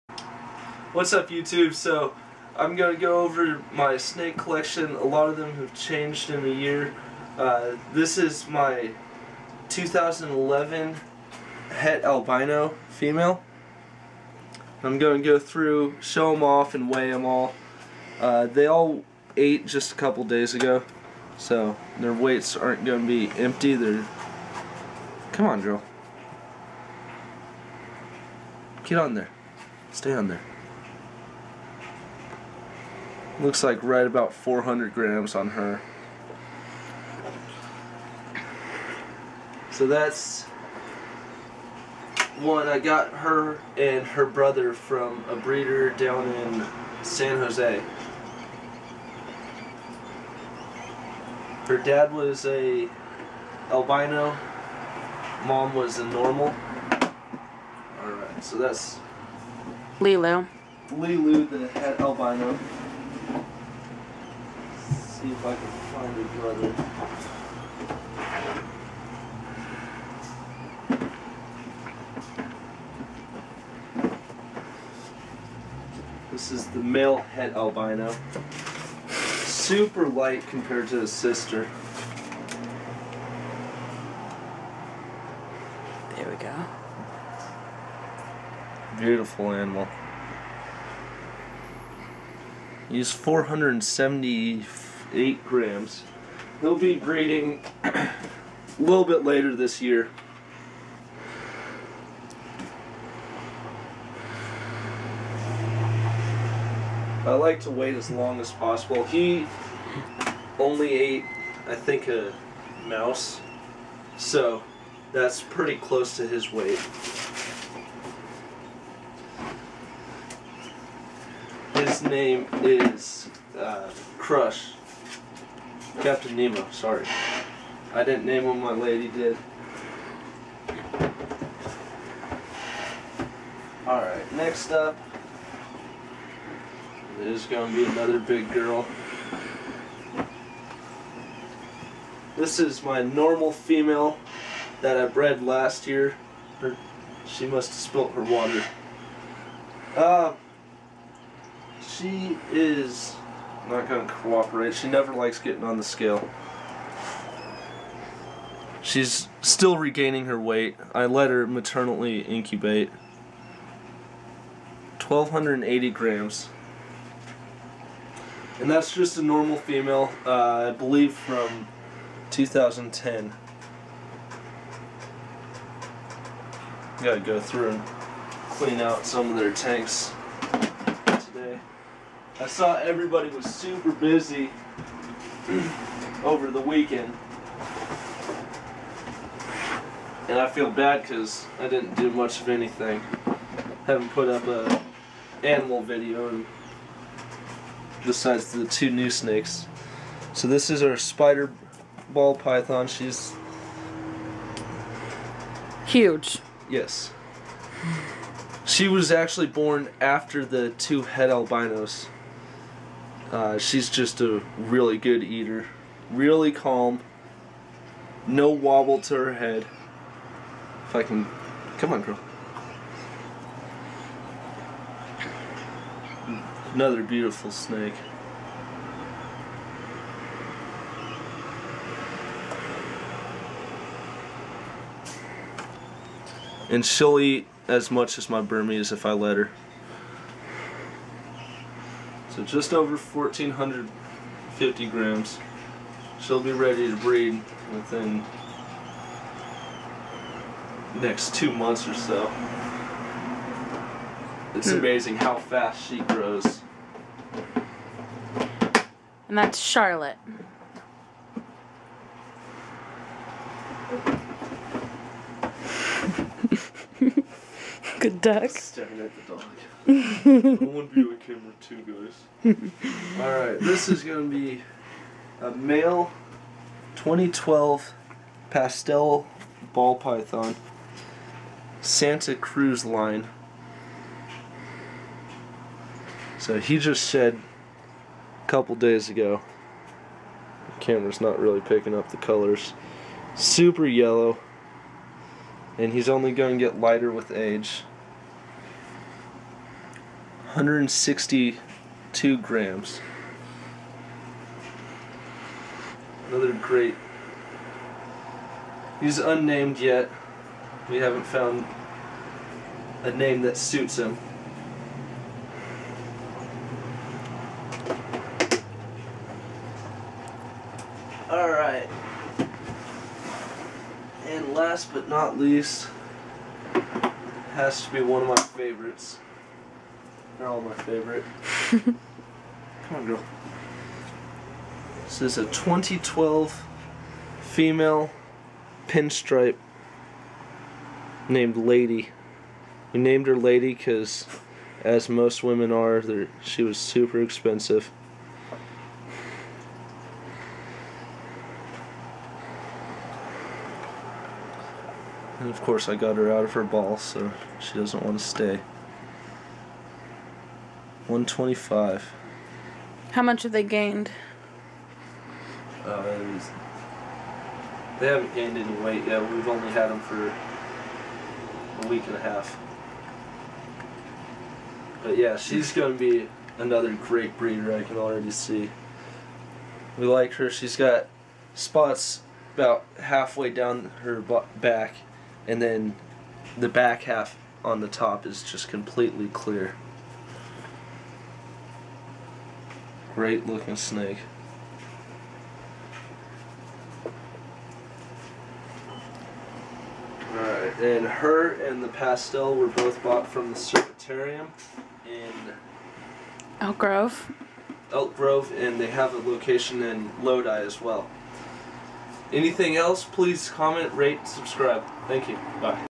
an up date on the snakes
In this video i go over my collection and wiegh them. A quick insight on my breeding plans.